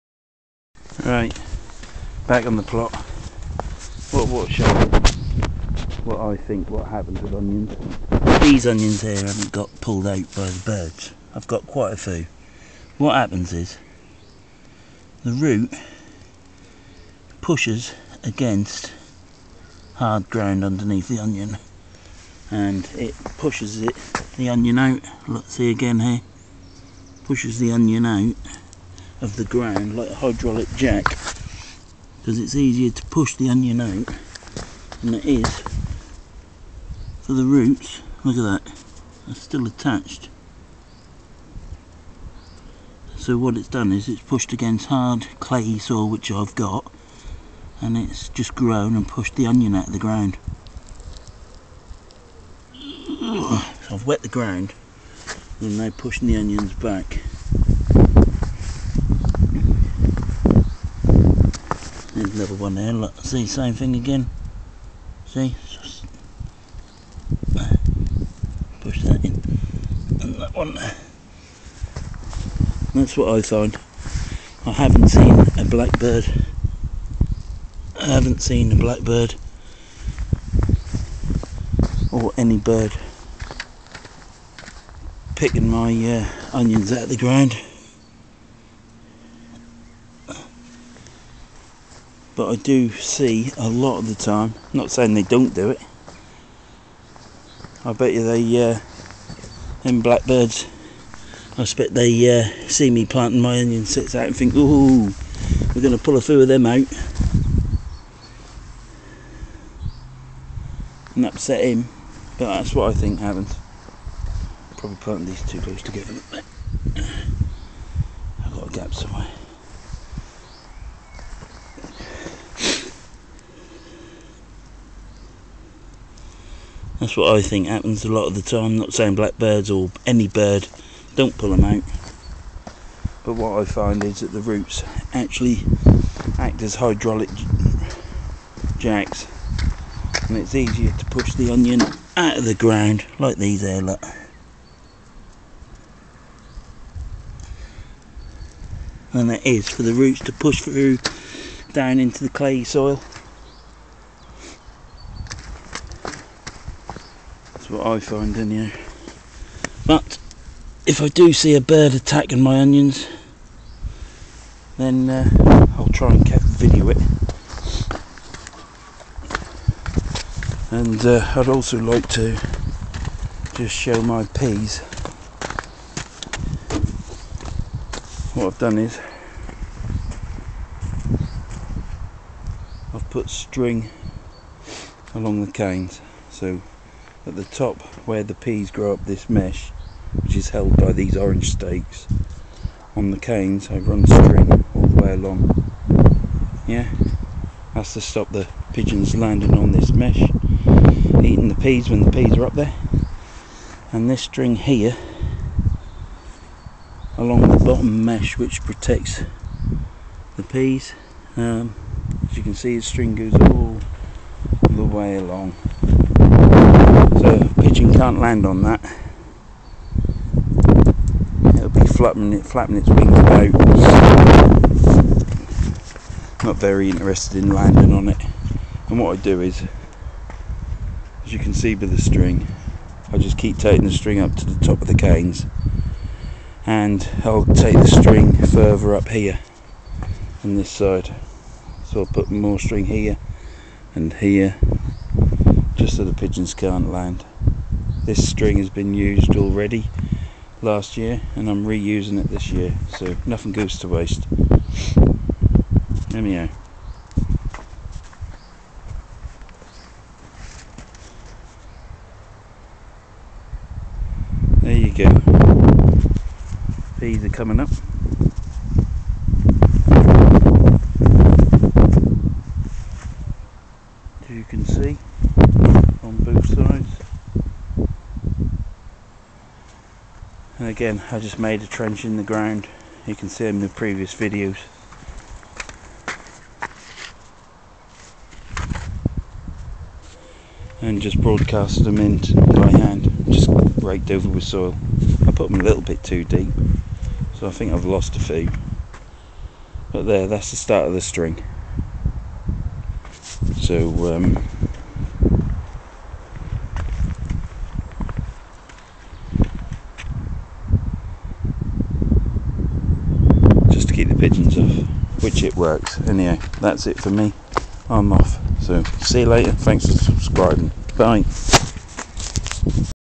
17/4/2016 onions trying to escape / peas
onions escaping from planting position not birds and string for peas